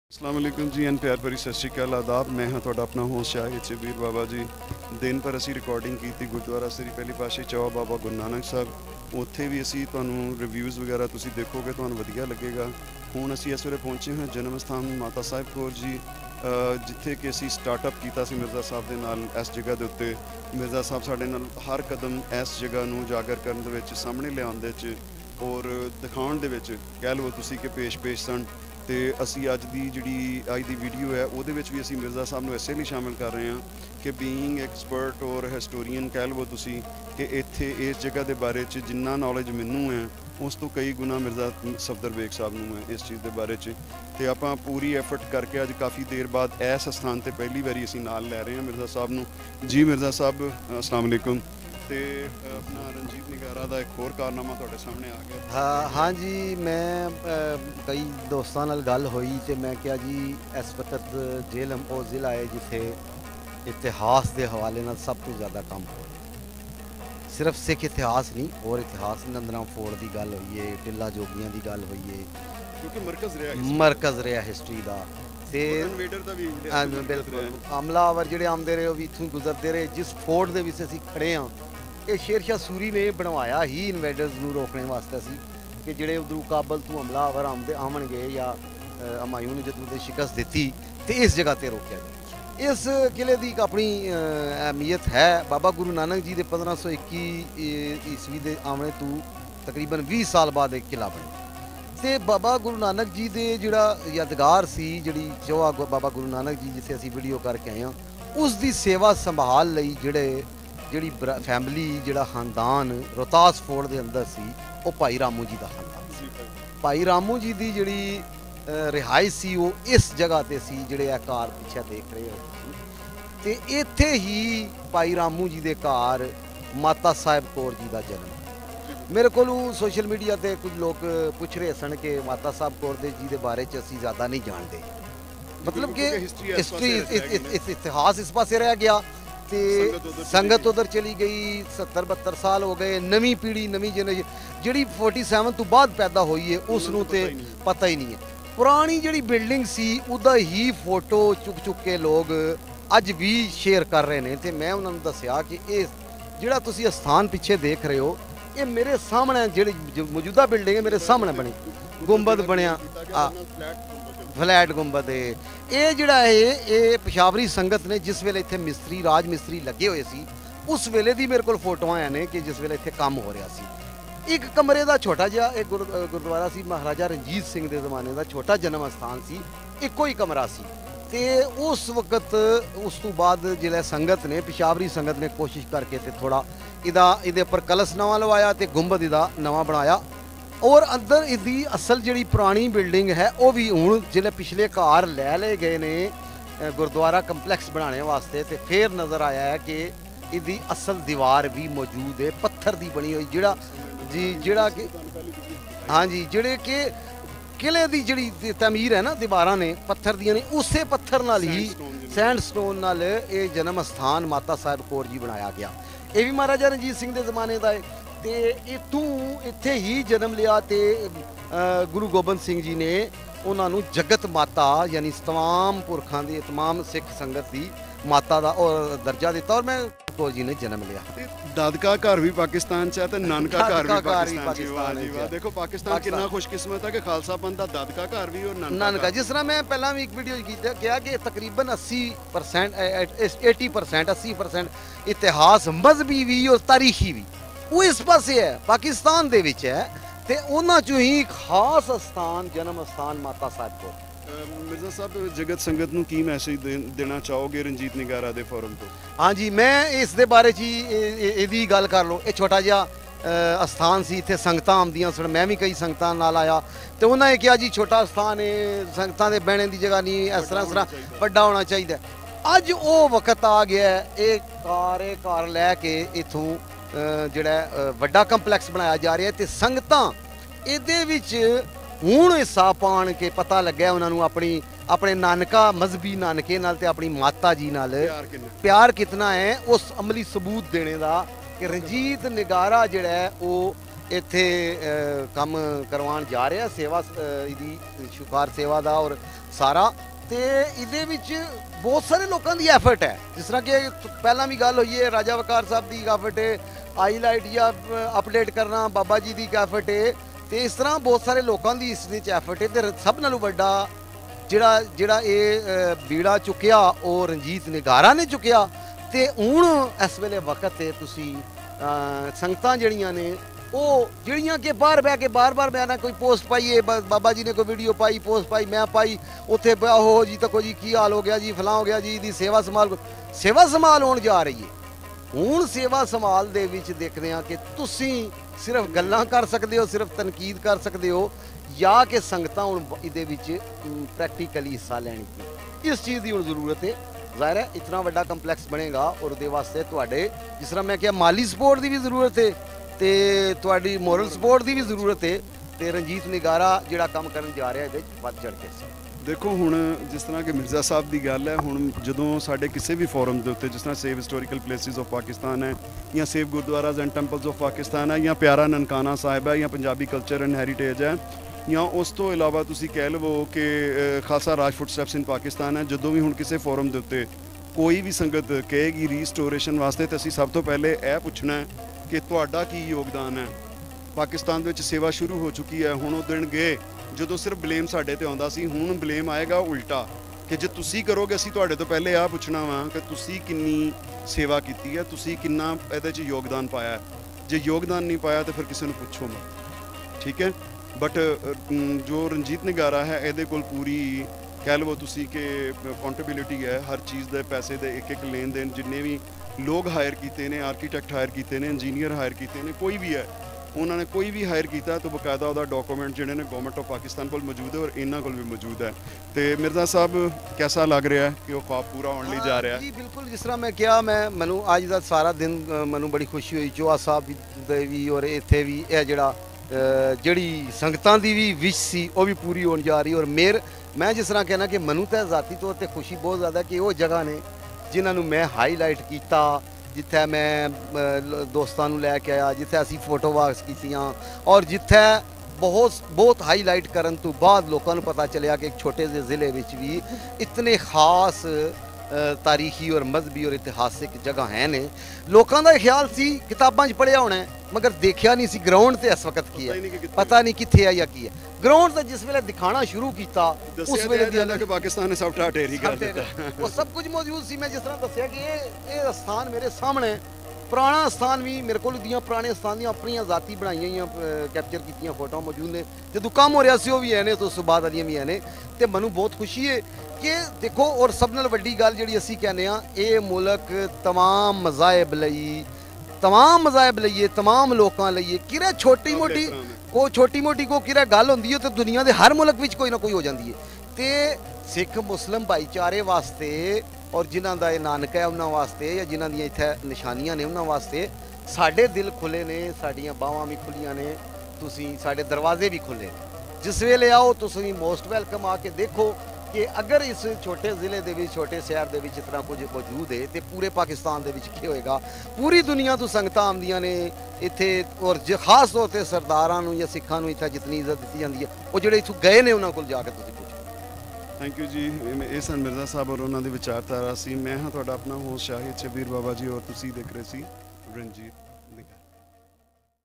Importance & Historical Background of Janam Asthan Mata Sahib Kaur ji Rohtas Fort
Importance & Historical Background of Janam Asthan Mata Sahib Kaur ji Rohtas Fort Tehsiel Dina District Jhelum North West Punjab Pakistan.